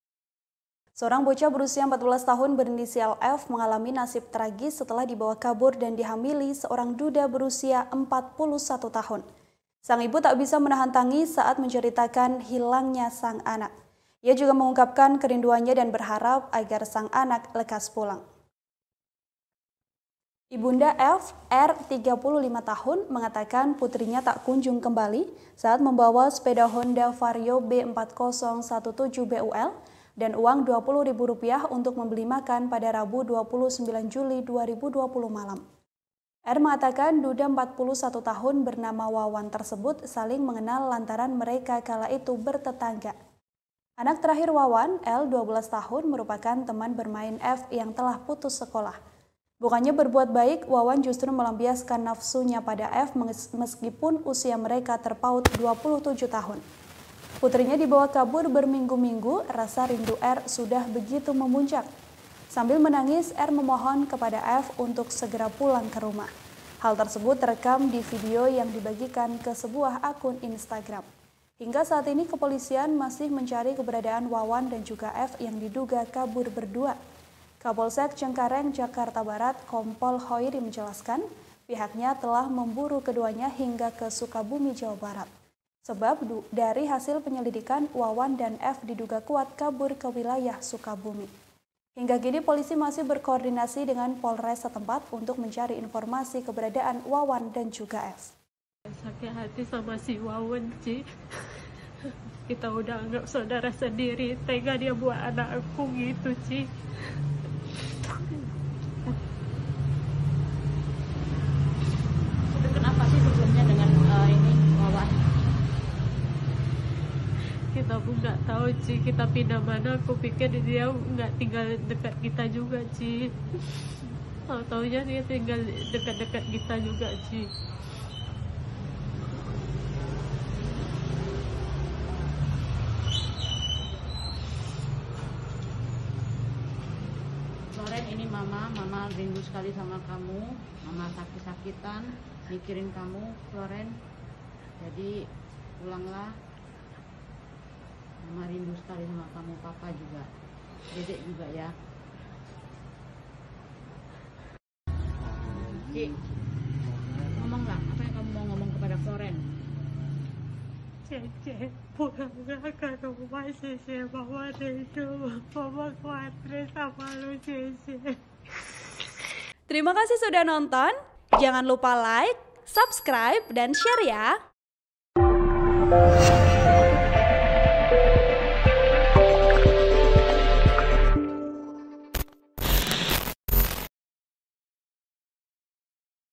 Pengakuan dan Tangis Ibu Bocah 14 tahun yang Anaknya Dibawa Lari Duda: Pulang Nak, Mama Rindu
TRIBUN-VIDEO.COM - Seorang bocah berusia 14 tahun berinisial F mengalami nasib tragis setelah dibawa kabur dan dihamili seorang duda berusia 41 tahun.

Sang ibu tak bisa menahan tangis saat menceritakan hilangnya sang anak.

Ia juga mengungkapkan keriduannya dan berharap agar sang anak lekas pulang.

Ibunda F, R (35), mengatakan putrinya tak kunjung kembali saat membawa motor Honda Vario B 4017 BUL dan uang Rp 20.000 untuk membeli makan pada Rabu (29/7/2020) malam.

R mengatakan, duda 41 tahun bernama Wawan tersebut saling mengenal lantaran mereka kala itu bertetangga.

Anak terakhir wawan, L (12), merupakan teman bermain F yang telah putus sekolah.

Bukannya berbuat baik, Wawan justru melampiaskan nafsuya pada F meskipun usia mereka terpaut 27 tahun.

Putrinya dibawa kabur berminggu-minggu, rasa rindu R sudah begitu memuncak.

Sambil menangis, R memohon kepada F untuk segera pulang ke rumah.

Hal tersebut terekam di video yang dibagikan ke sebuah akun Instagram.

Hingga saat ini, kepolisian masih mencari keberadaan Wawan dan juga F yang diduga kabur berdua.

Kapolsek Cengkareng Jakarta Barat, Kompol Khoiri menjelaskan, pihaknya telah memburu keduanya hingga ke Sukabumi, Jawa Barat.

Sebab, dari hasil penyelidikan, Wawan dan F diduga kuat kabur ke wilayah Sukabumi.

Hingga kini, polisi masih berkoordinasi dengan Polres setempat mencari informasi Wawan dan F.(*)